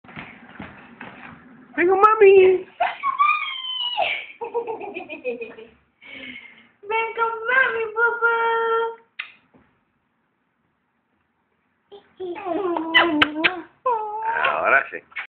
Brandon Lee Sanchez
Brandon walking all over the house and getting into everything now!